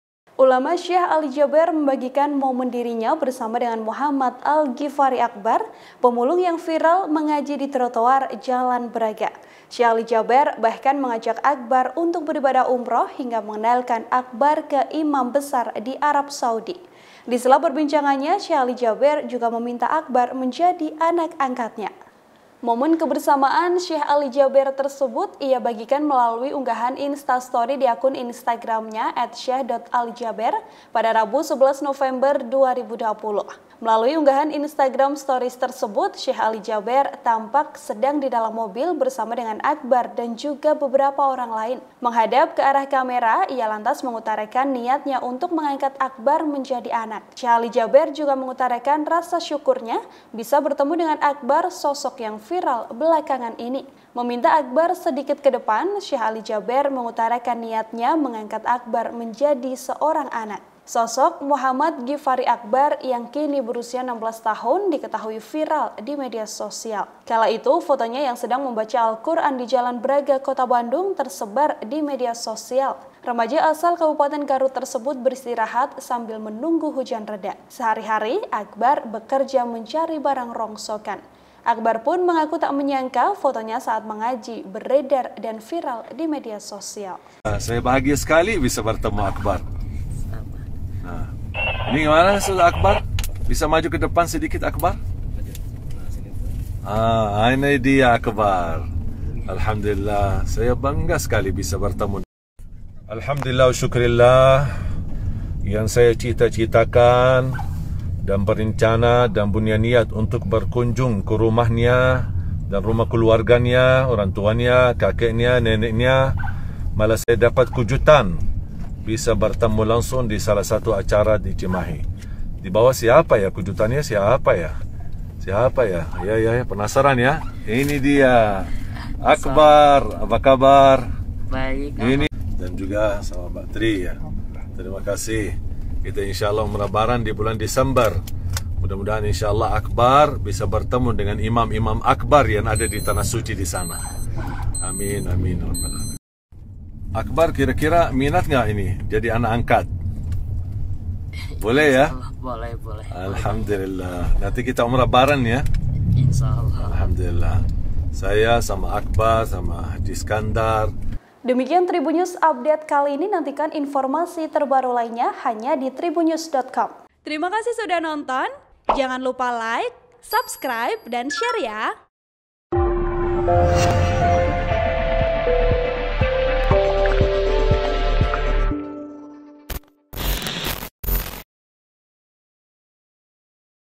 Akbar yang Viral Ngaji di Emperan Bertemu Syekh Ali Jaber, Akan Diangkat Anak dan Diajak Umroh
TRIBUN-VIDEO.COM - Ulama Syekh Ali Jabar, membagikan momen dirinya bersama dengan Muhammad Al Gifari Akbar, pemulung yang viral mengaji di Trotoar, Jalan Braga.

Syekh Ali Jaber bahkan mengajak Akbar untuk beribadah umroh hingga mengenalkan Akbar ke imam besar di Arab Saudi.

Di sela pembicaraannya, Syekh Ali Jaber juga meminta Akbar menjadi anak angkatnya.

Momen kebersamaan Syekh Ali Jaber tersebut, ia bagikan melalui unggahan Instastory di akun Instagramnya @syekh.alijaber, Rabu (11/11/2020).

Melalui unggahan Instagram stories nya, Syekh Ali Jaber tampak sedang di dalam mobil bersama dengan Akbar dan juga beberapa orang lain.

Menghadap ke kamera, ia lantas mengutarakan niatnya untuk mengangkat Akbar menjadi anak.

Syekh Ali Jaber juga mengutarakan rasa syukurnya, bisa bertemu dengan Akbar, sosok yang viral belakangan ini.

"Kita mau umroh di bulan Desemberm mudah-mudahan Akbar bisa bertemu dengan Imam-imam Akbar yang ada di tanah suci sana," jelas Syekh Ali Jaber.

Meminta Akbar sedikit ke depan, Syekh Ali Jaber lantas mengutarakan niatnya mengangkat Akbar menjadi anak.

"Akbar minat enggak ini, jadi anak angkat, boleh ya," ucap Syekh Ali Jaber lagi.

Remaja asal Kabupaten Garut itu beristirahat sambil menunggu hujan reda.

Sehari-hari Akbar diketahui bekerja mencari barang rongsokan.

(Tribun-Video.com/Nila)

Artikel ini telah tayang di KompasTV dengan judul 'Syekh Ali Jaber Minta Akbar Pemulung Jadi Anak Angkat'